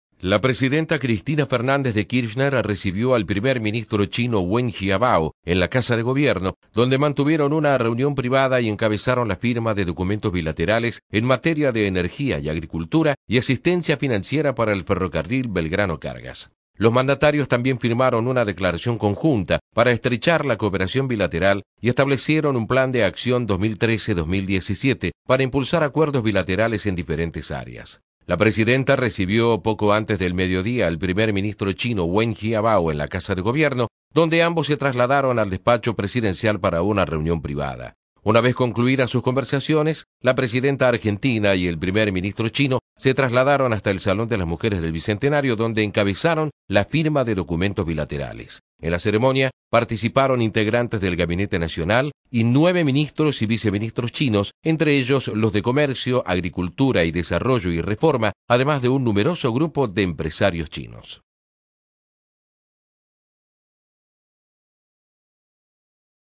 Cristina y Jiabao firmaron acuerdos bilaterales
La presidenta Cristina Fernández de Kirchner recibió al Primer ministro chino Wen Jiabao en la Casa de Gobierno, donde mantuvieron una reunión privada y encabezaron la firma de documentos bilaterales en materia de Energía y Agricultura y asistencia financiera para el ferrocarril Belgrano.

Los mandatarios también firmaron  una Declaración Conjunta para estrechar la cooperación bilateral y establecieron un plan de acción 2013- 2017 para impulsar acuerdos bilaterales en las distintas áreas.

La presidenta Cristina Fernández de Kirchner recibió a las 11:20 al primer ministro chino Wen Jiabao en la Casa de Gobierno, donde ambos se trasladaron al despacho presidencial para una reunión privada

Una vez concluida sus conversaciones, Cristina y Wen Jiabao se trasladaron  hasta el Salón de las Mujeres del Bicentenario, para encabezar la firma de documentos bilaterales.
En la ceremonia participaron integrantes del gabinete nacional y nueve ministros y viceministros chinos, entre ellos los de Comercio, Agricultura y Desarrollo, y  Reforma, además de un numeroso grupo de empresarios chinos.